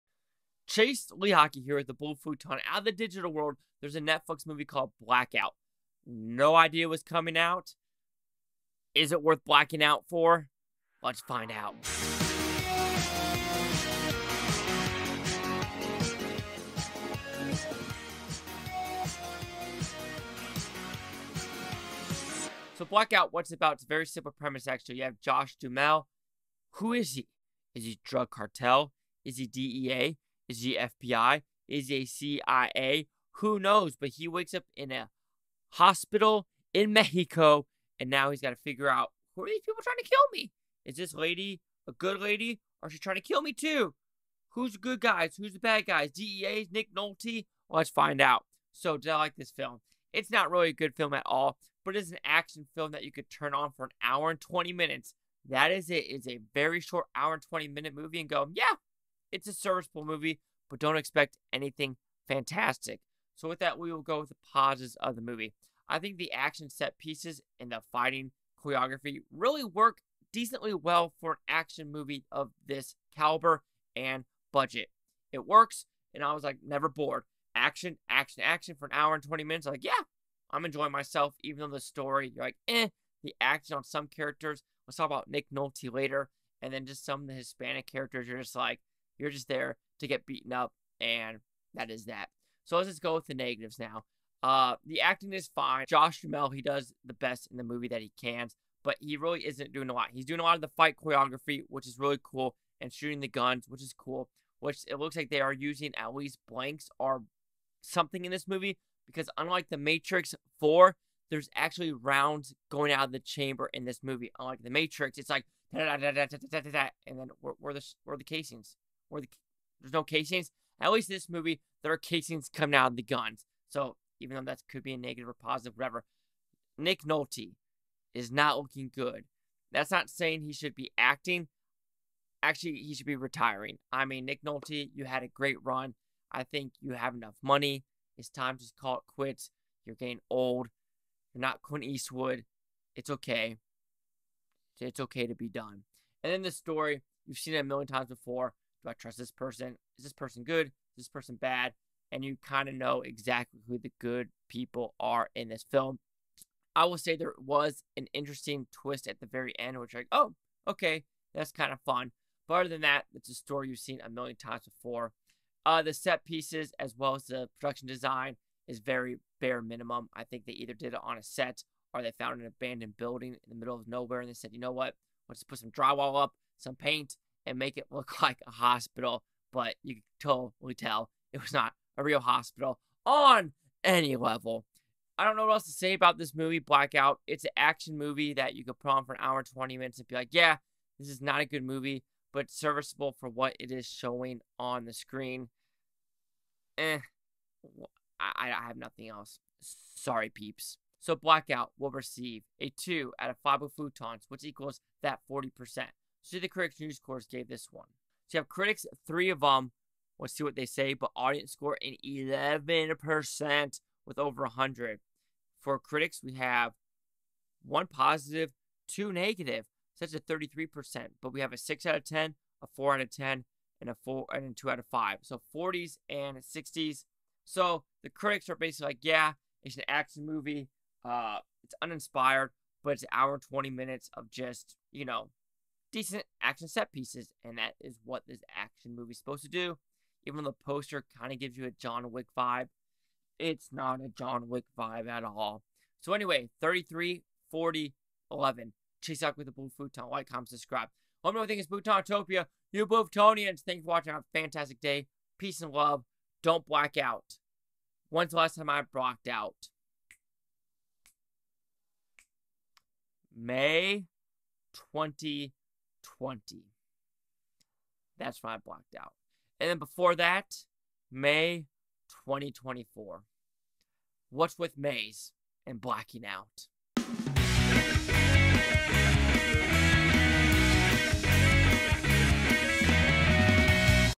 Blackout Movie Review--Pros and Cons of Blacking Out
Blackout (2022) is directed by Sam Macaroni & stars Josh Duhamel, Abbie Cornish, Omar Chaparro, Bárbara de Regil, Hernan Del Riego, Edison Ruíz, Daniel Farag, Lou Ferrigno Jr. and Nick Nolte

TV-MA